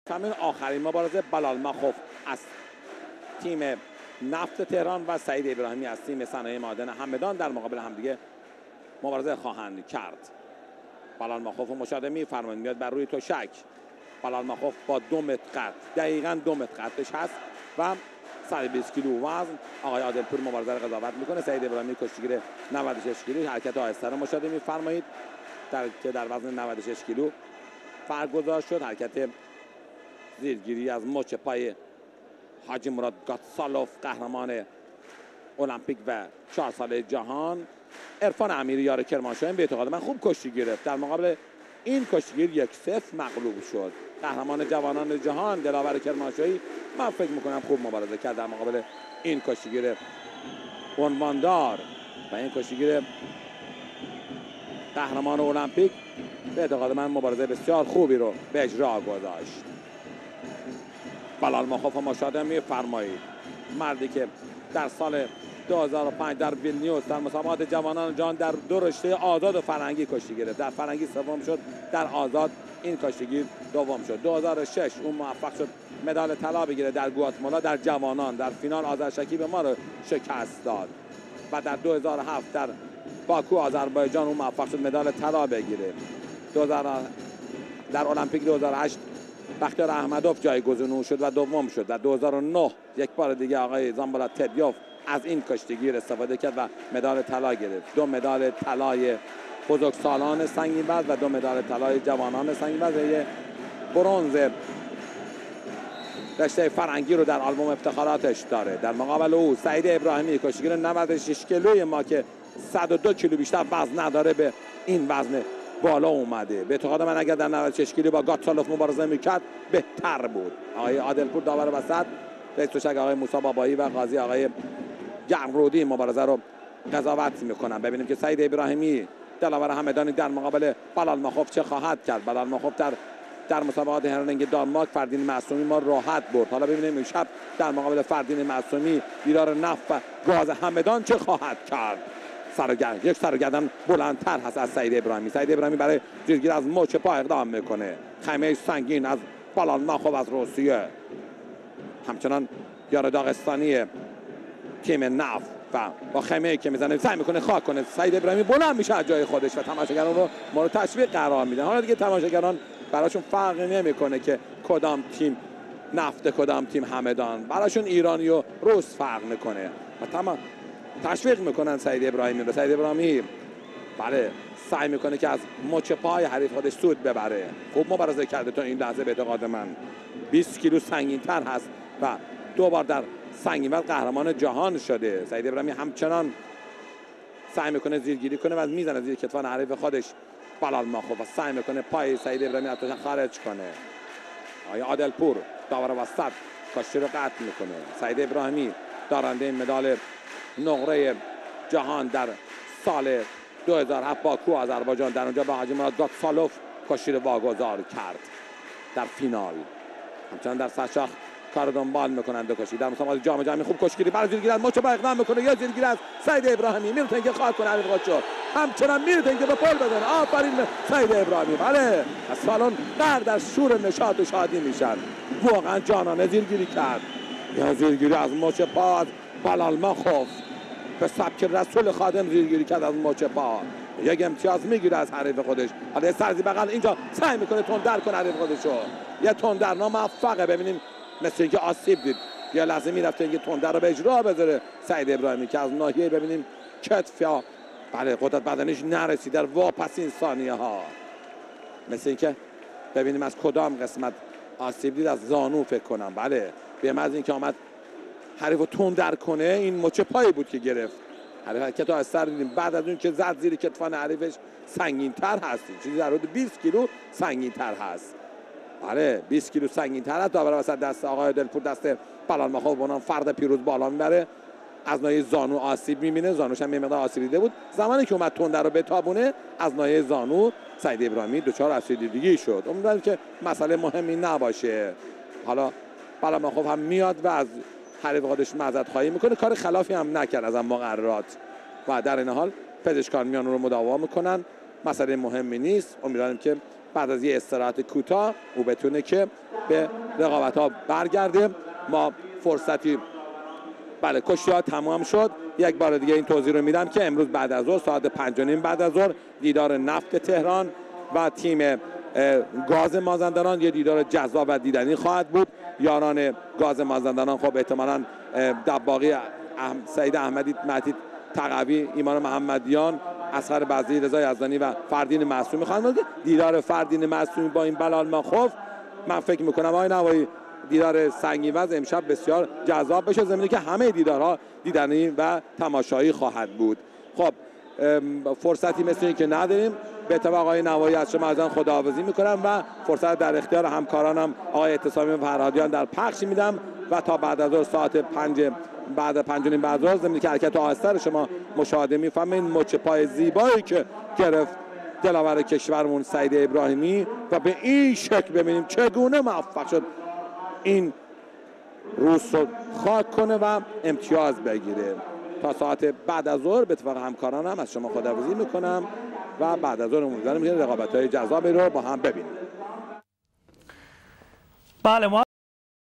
Saeid Ebrahimi vs. Bilyal Makhov
Iranian freestyle wrestling league, 18 Jan 2010, 120 kg semifinal: Saeid Ebrahimi vs Bilyal Makhov.

105-кг Эб делает бросок 120-кг любимцу Саламбека :)